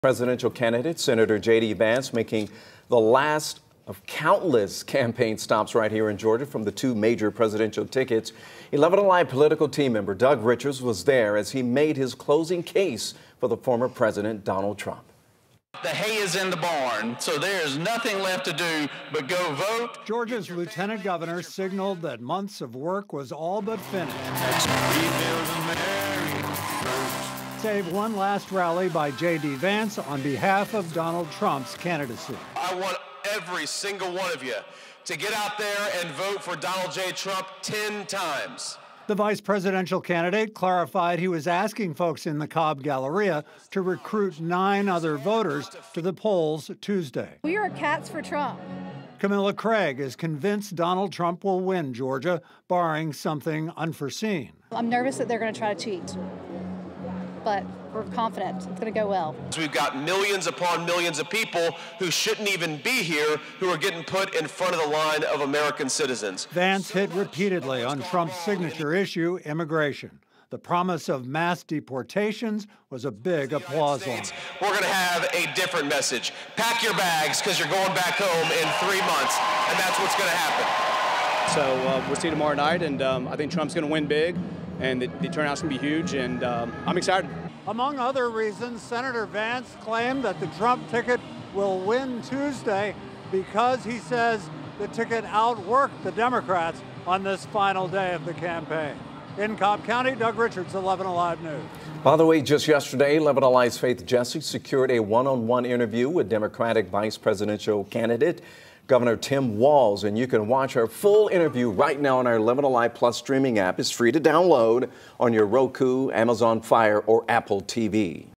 On the eve of Election Day, JD Vance makes the case for Donald Trump in Georgia
During his speech, Vance repeatedly hit on Trump's signature issue: immigration.